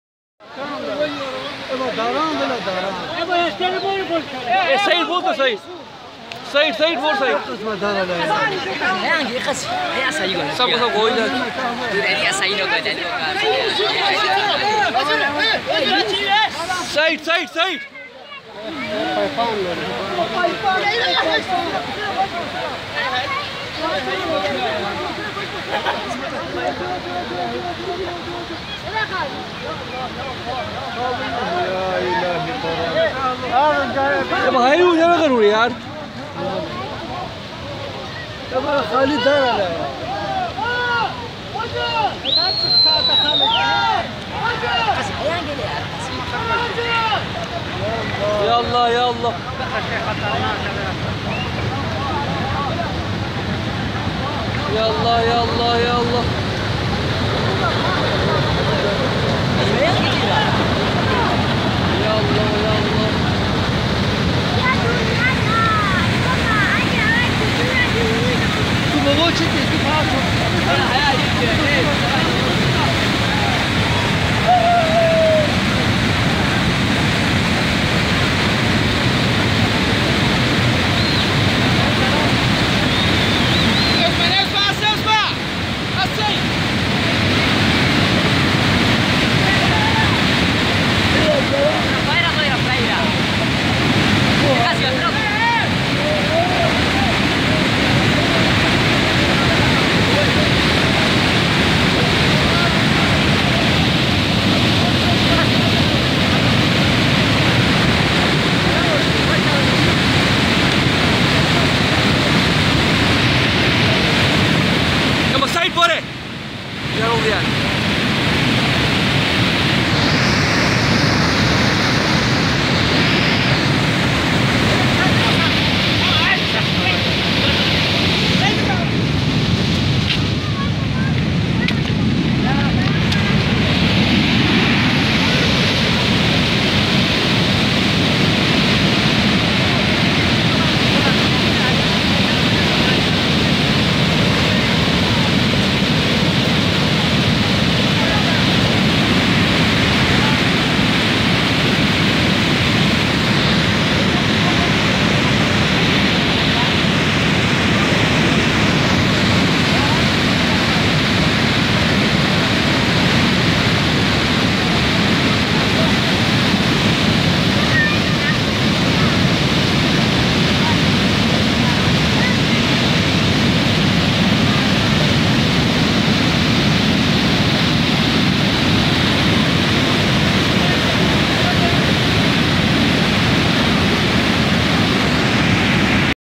Flooding in chitral 2015
Video of disastrous flooding in chitral Pakistan